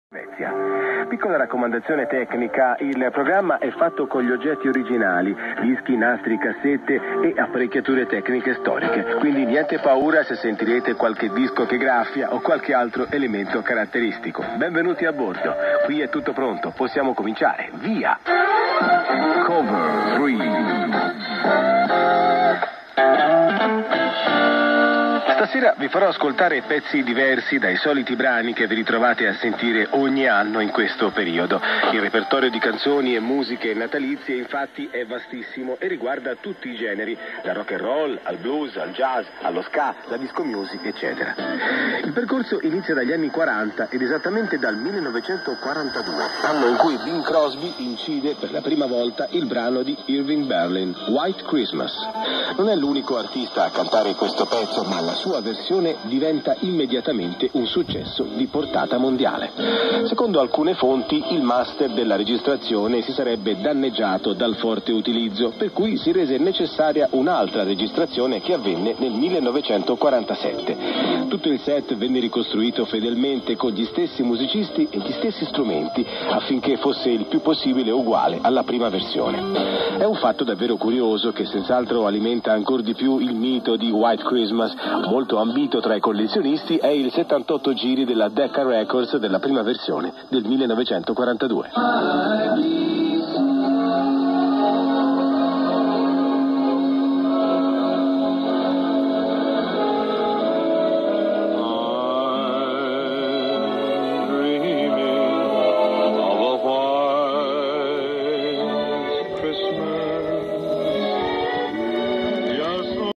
Frequency of 6070 kHz - IBC Radio
IBC Radio via Rohrbach Waal, 25 kW. A lovely reception of my favourite program.